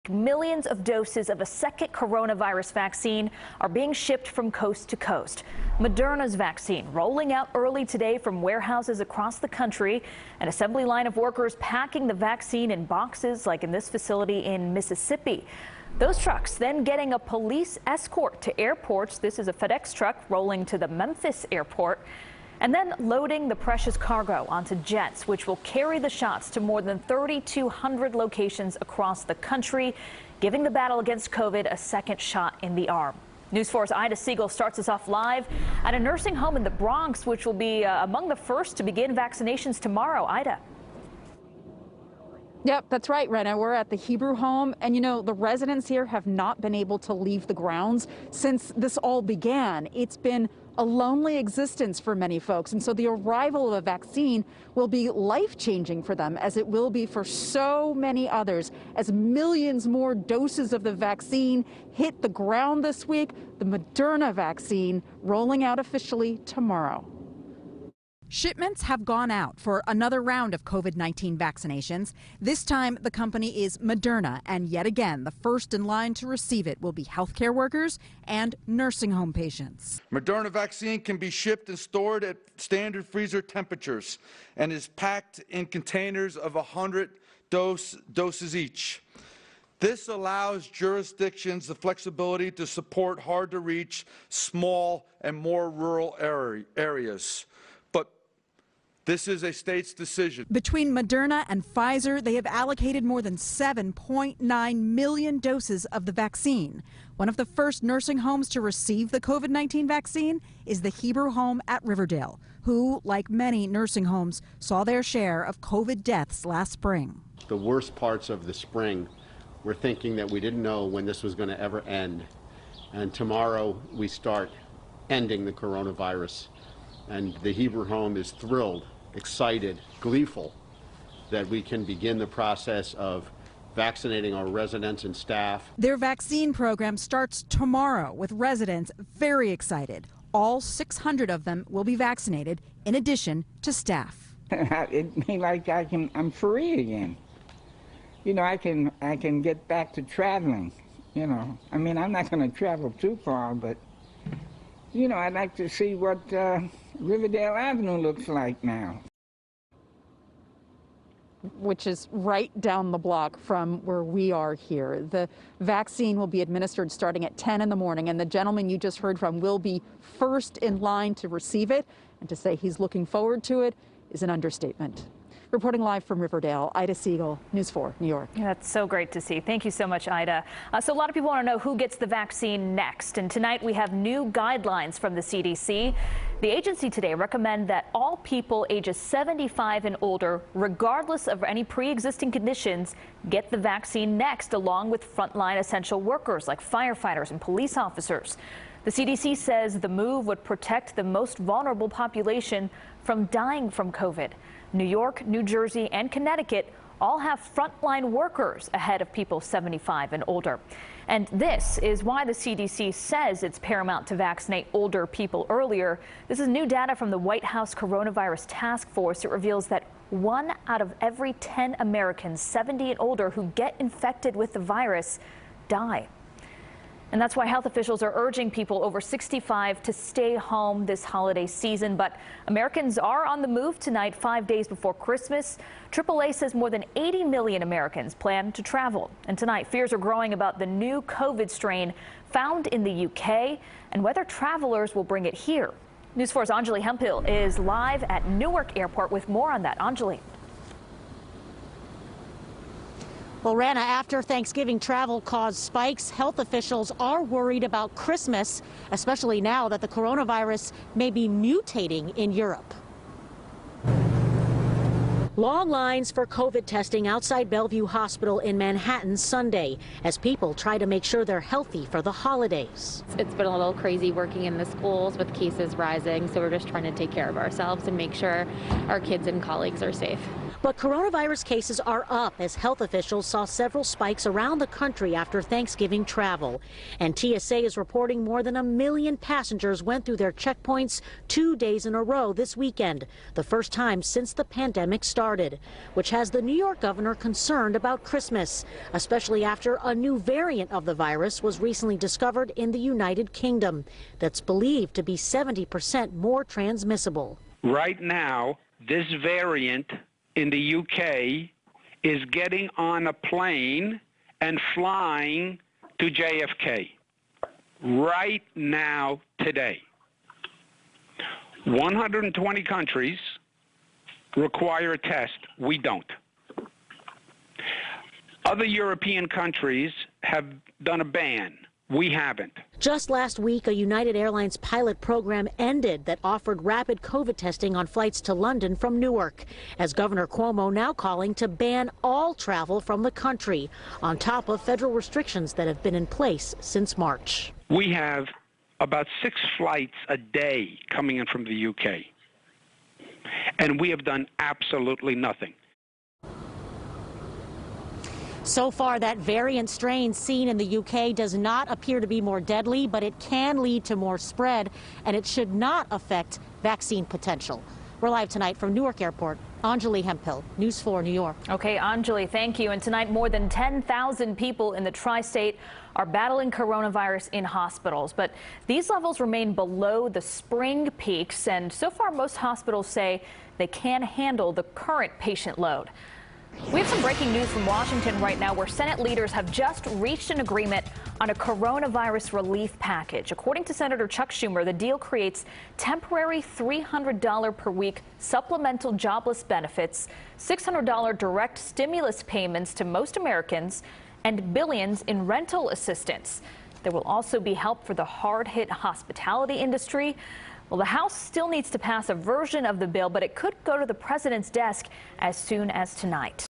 Covid-19 Mutation in Europe Causes New Travel Fears | NBC New York
Millions more doses of a coronavirus vaccine shipped out to states on Sunday as new concerns of a mutation in Europe lead to travel bans ahead of the Christmas holiday. NBC New York’s Ida Siegal and Anjali Hemphill report.